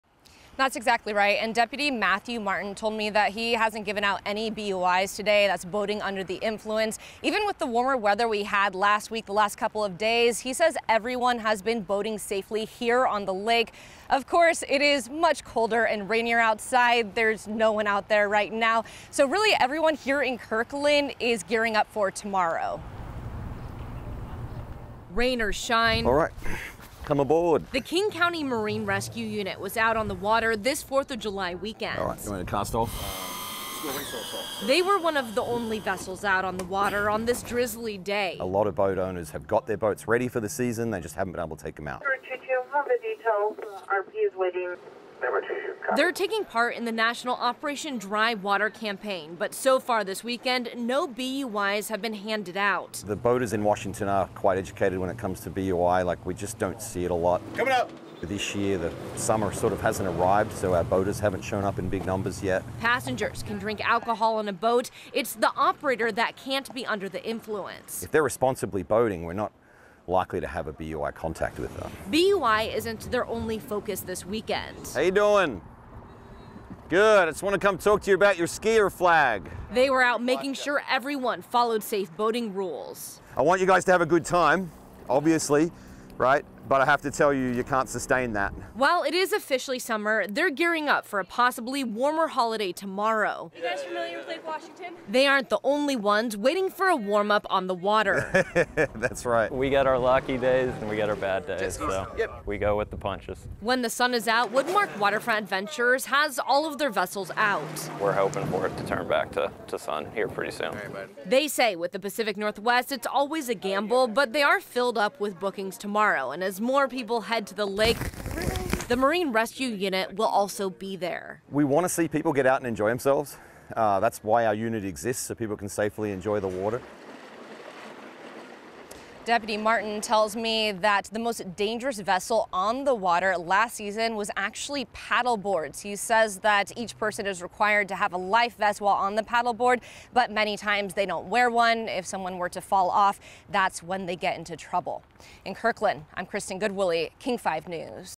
Slow start for western Washington boaters this Fourth of July weekend
Operation Dry Water is in effect through tomorrow. King County is increasing boat patrols to keep everyone safe from those boating under the influence.